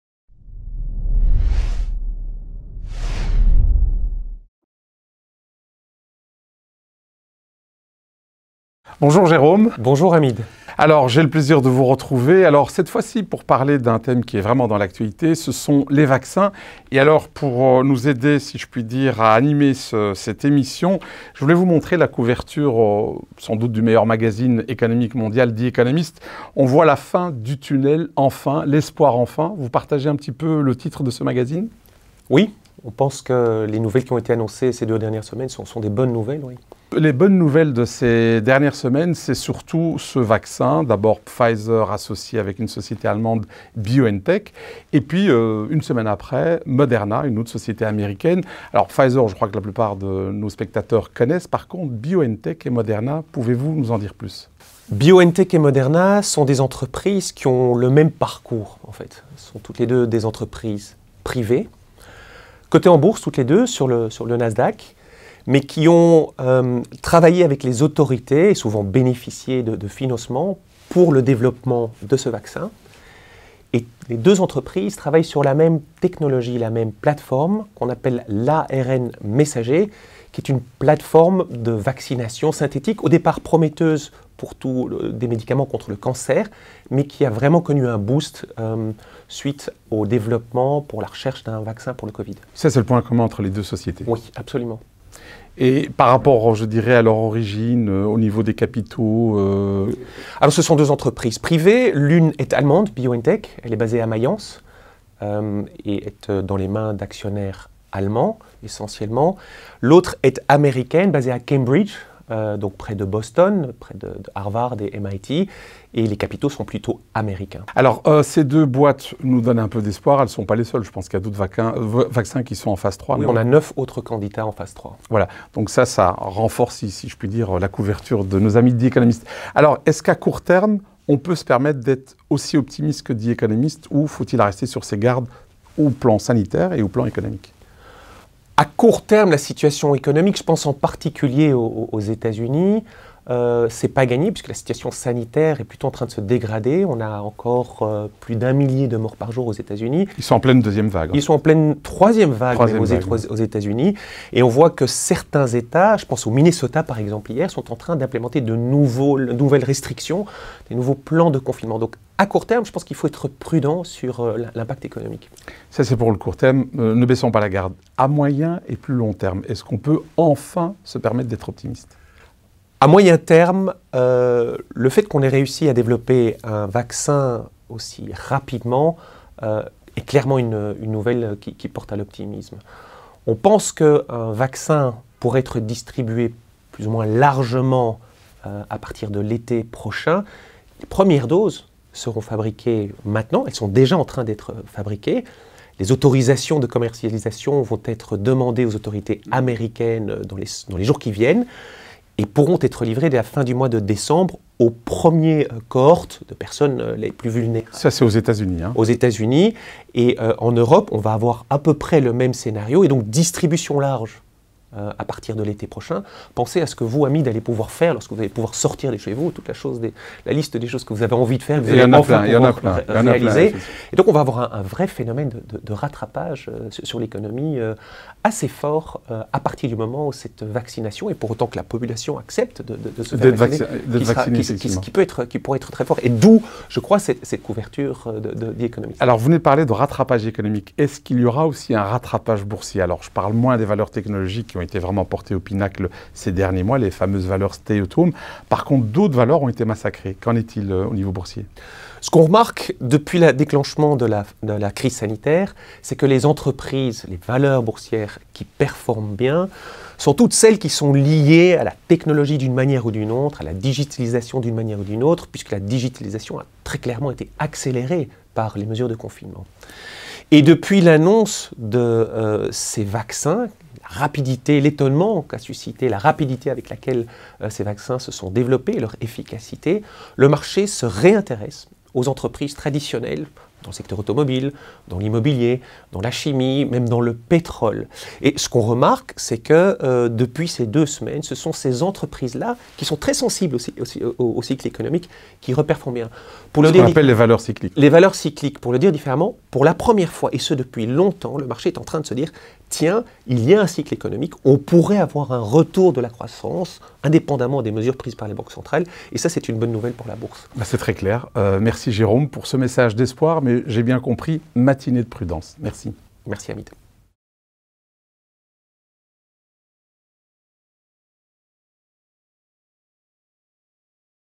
Le vaccin : le remède à la reprise économique ?
Les groupes pharmaceutiques Pfizer-BioNTech et Moderna ont annoncé à une semaine d’intervalle les résultats de tests cliniques pour leur vaccin contre le coronavirus. Les résultats sont excellents puisqu’on évoque une efficacité de plus de 90% de ce vaccin dans les deux cas, soit presque le double de celui de la grippe saisonnière.